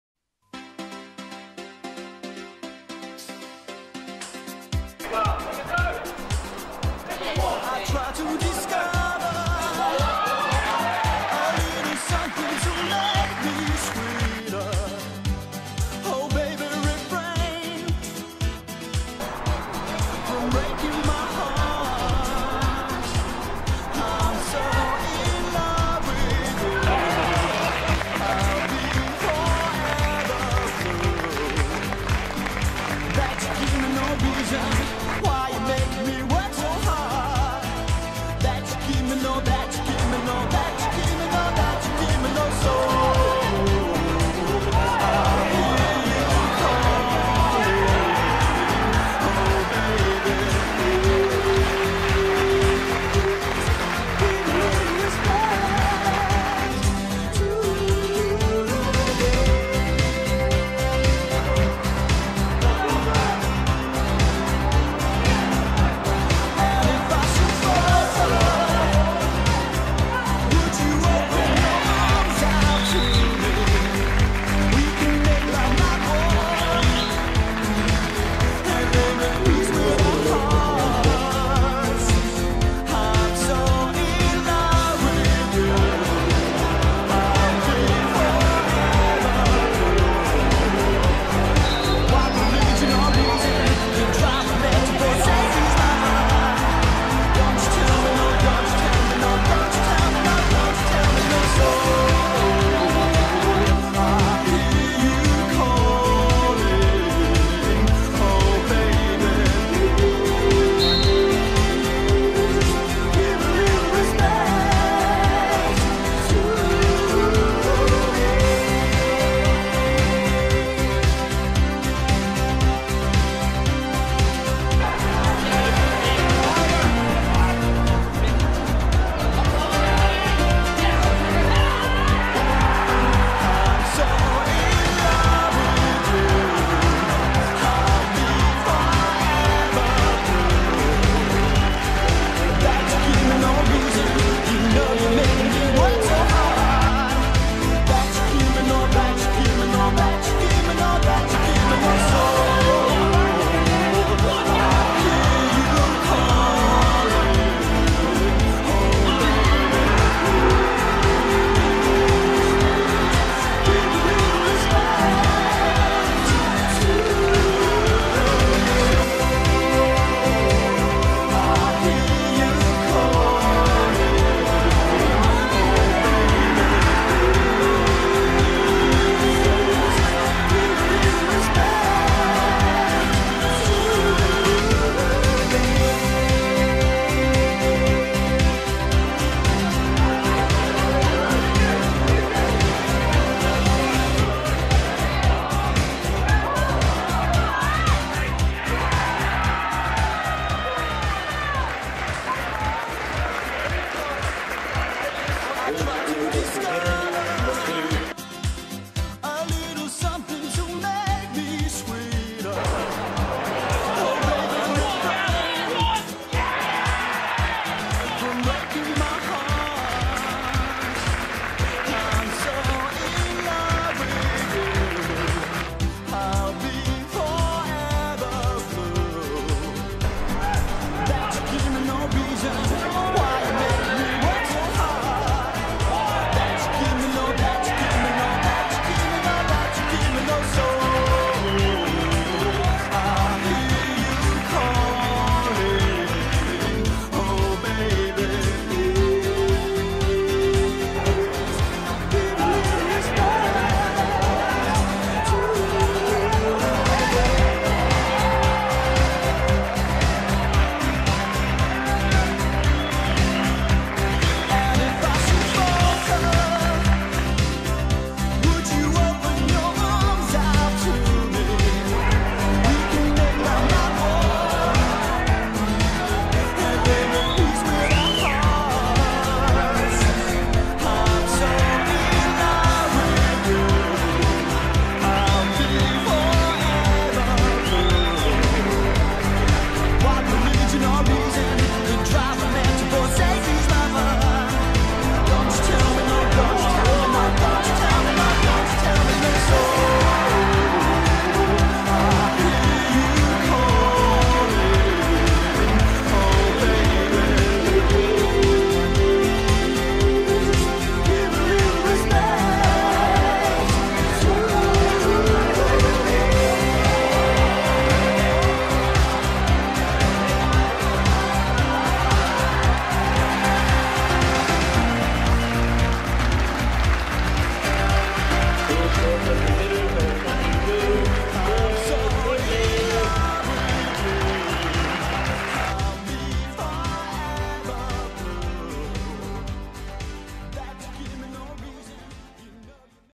CruesTV Goals of the Season
the best goals caught on camera at Seaview from the League winning season